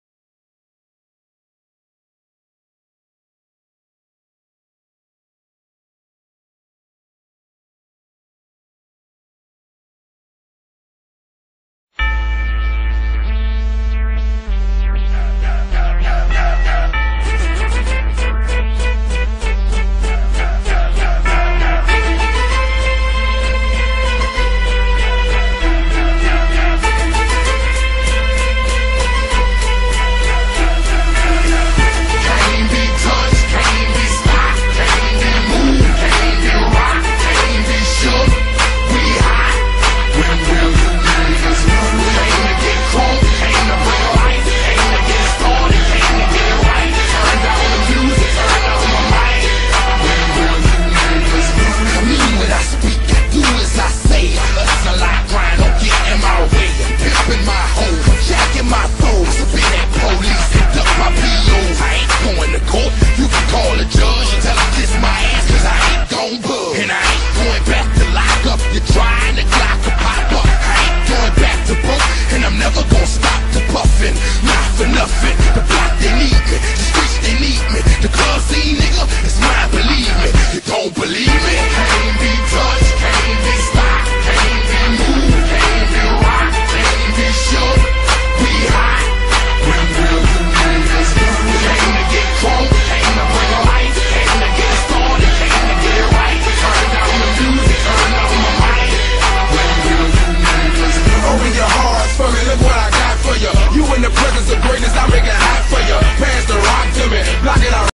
Cristiano Ronaldo * NeW 2010 *CR9 The Golden Boy 2010 [HD]
In my new video, I hope it's in HD, as always represent Cristiano Ronaldo (Real Madrid CF), with its inevitable tricks of legs I hope you like my video ... A greeting from the staff of CR9RONALDO9